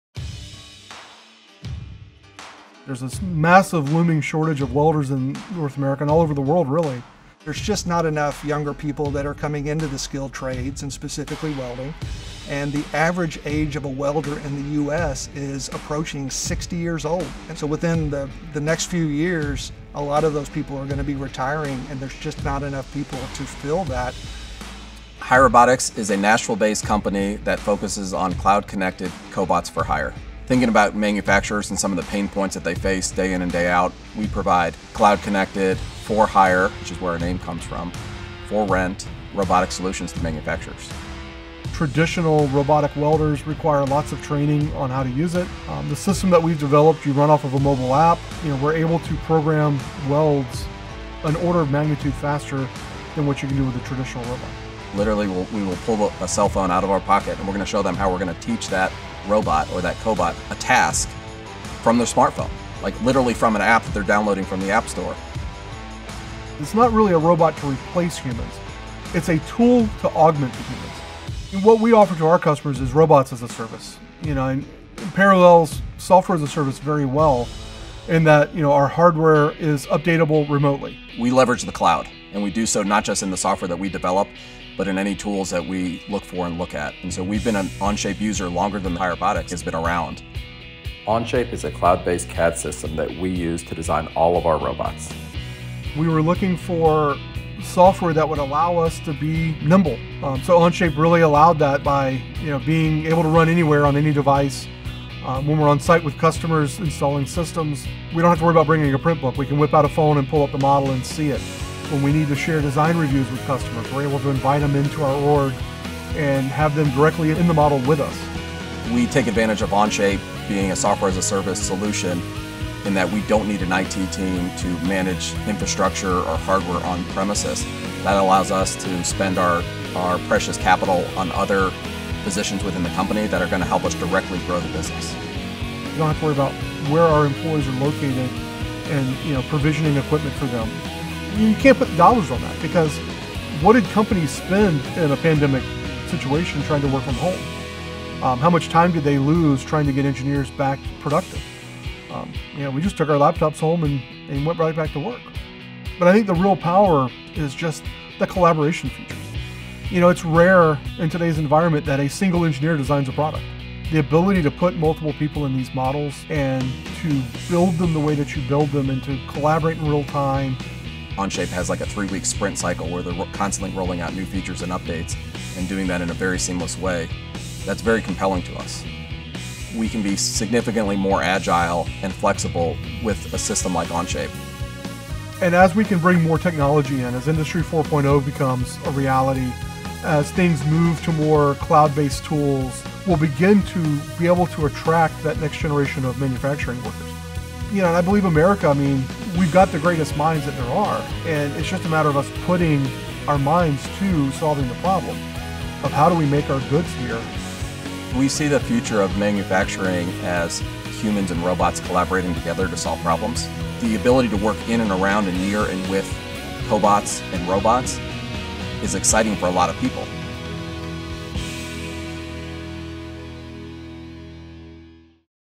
Makers Series | Hirebotics, powered by PTC’s Onshape
Meet Rob Goldiez and Matthew Bush, co-founders of Hirebotics. In this edition of NAM’s Makers Series, Goldiez and Bush explain how Hirebotics uses PTC’s Onshape cloud-based CAD system to power its design teams.

CDC-recommended protocols were observed in filming this video.